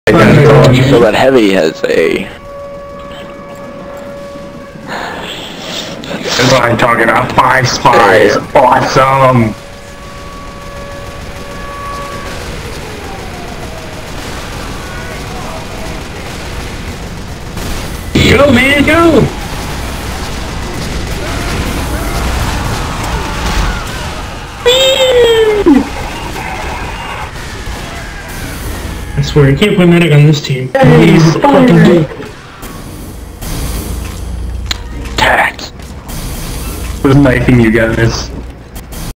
5 Spies Dominate the ENTIRE Blue Team
Just a funny replay I captured on No Heroes Payload Race Server.

My team had 5 spies and we decided to go and completely wreck the entire blue team for the lolz. This is why I love servers without class limiters, because it means hilarious stuff like this can happen.

Also, enjoy the alltalk.

You can find me on steam as SubrosianDimitri

Join my steam group, the Syndicate of Unlucky Spies if you want to shove a knife up lady luck's ***